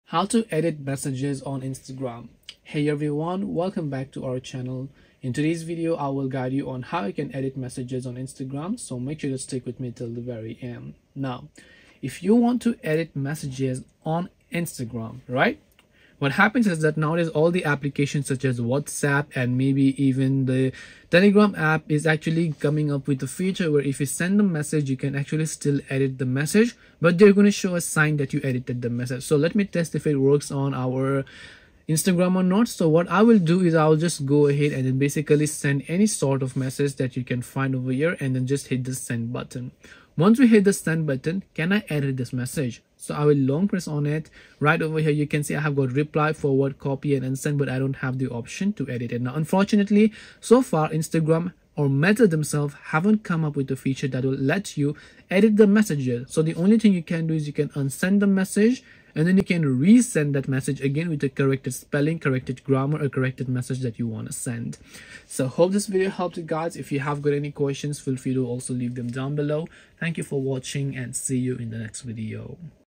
How to Edit Messages on Instagram
This video guides you through an easy step-by-step process to edit messages on Instagram. So make sure to watch this video till the end.

~ Chapters:
0:00 Introduction
0:11 Edit Messages on Instagram
1:05 Conclusion